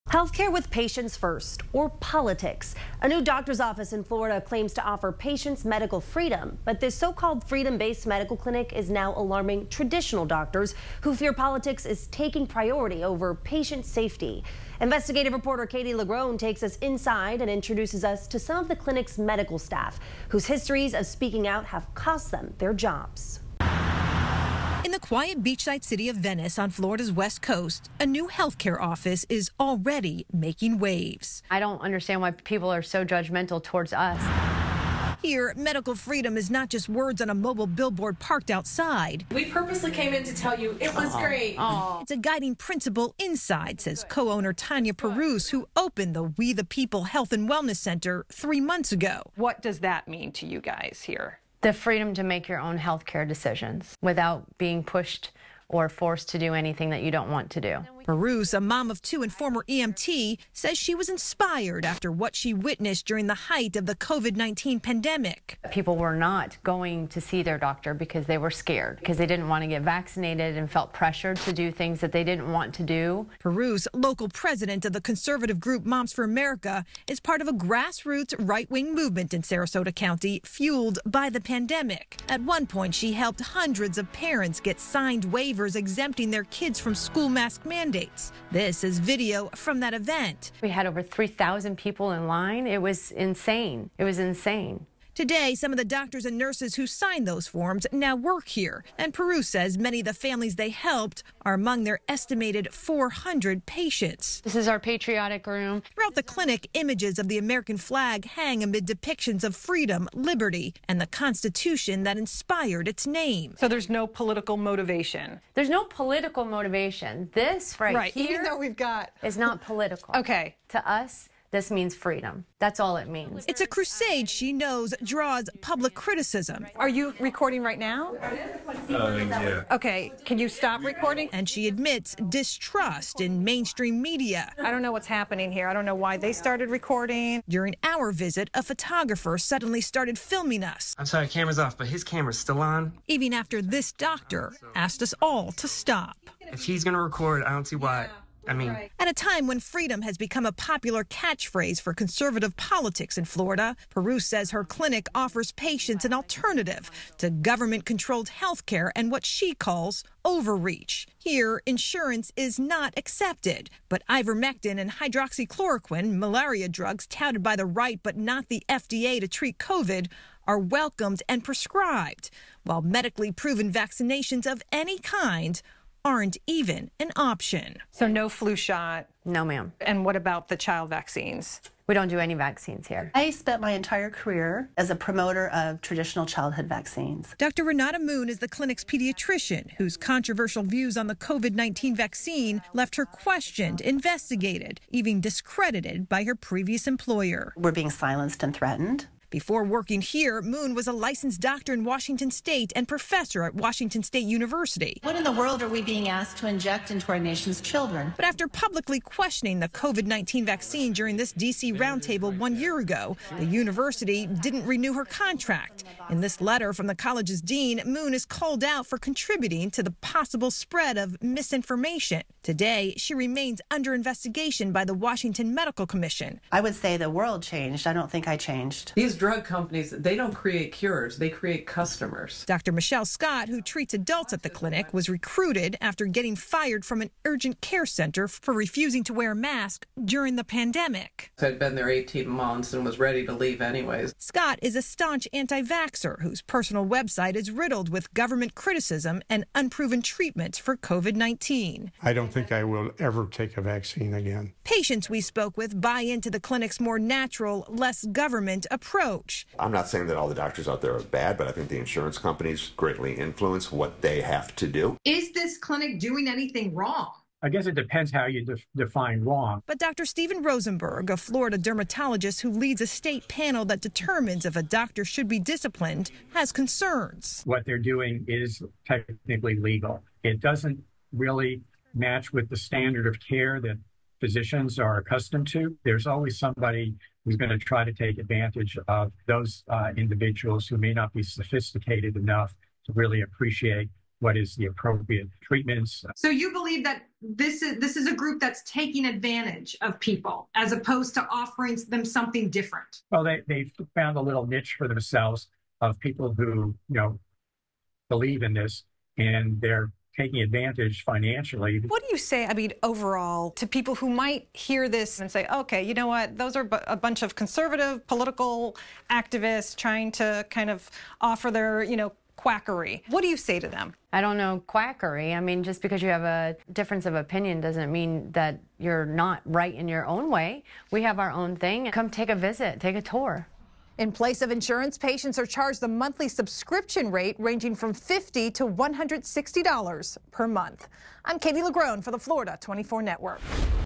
'Freedom-based' health care clinic concerns mainstream medical community
A new "freedom-based" clinic opened in Venice, Florida, a few months ago.  Investigative Reporter Kaite LaGrone takes you inside and introduces you to its doctors who have lost jobs.